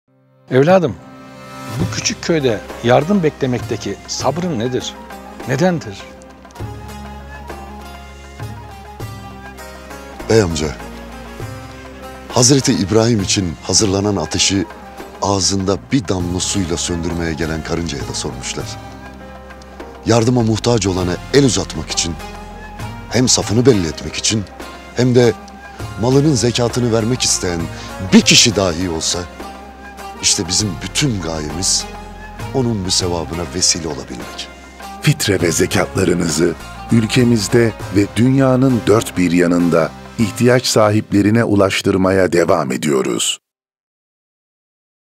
Türkiye Diyanet Vakfı Zekat Tanıtım Filmi 2023 - Kamu Spotu
Hz. İbrahim'e (as) su taşıyan karınca misali kardeşlerimizin yardımına koşuyoruz.

Yıl boyu yapacağınız yardımlarınızla

ZEKAT yazıp 5601'e SMS göndererek 25 TL bağışta bulunabilir, bagis.tdv.org adresinden online bağış yapabilirsin.